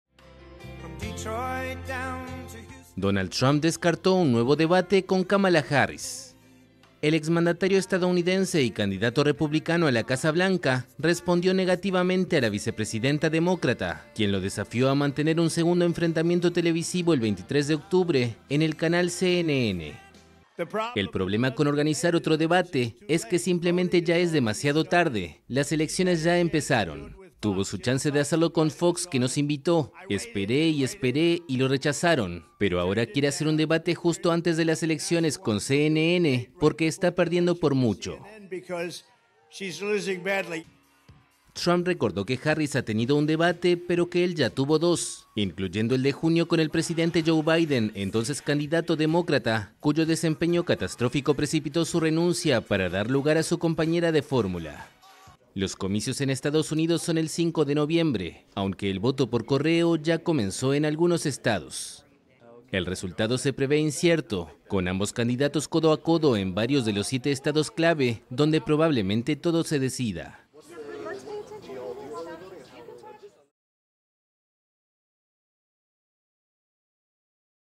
Trump dice que es "demasiado tarde" para otro debate con Harris en EUA
El expresidente estadounidense Donald Trump dijo el sábado que es "demasiado tarde" para organizar un nuevo debate con su rival en las elecciones del 5 de noviembre, la demócrata Kamala Harris, que volvió a desafiarlo a mantener un segundo enfrentamiento televisivo.